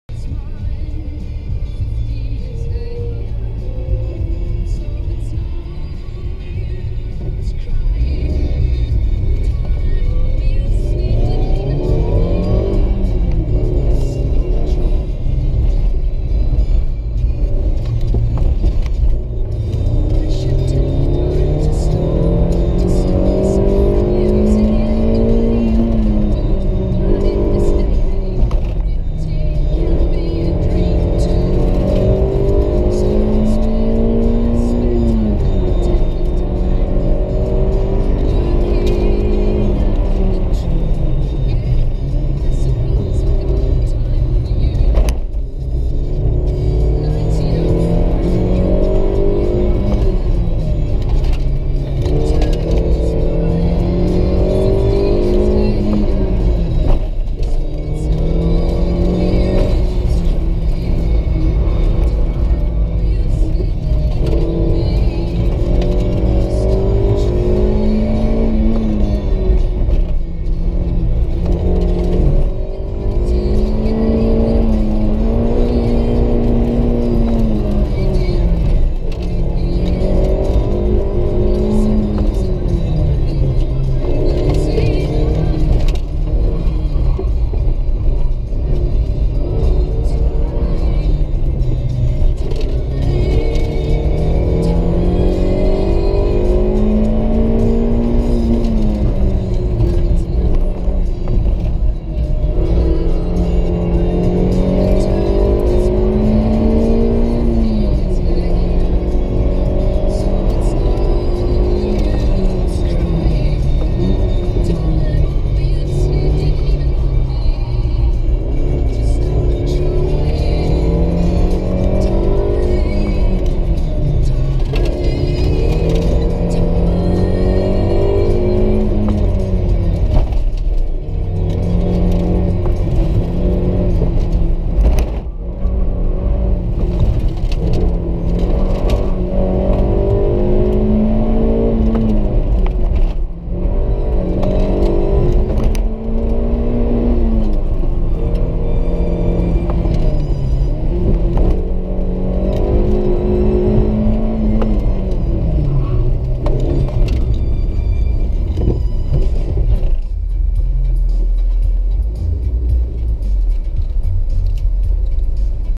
Manfeild Backtrack Autocross Run 1
Having a slide around on the dust and mud, in the Swift Sport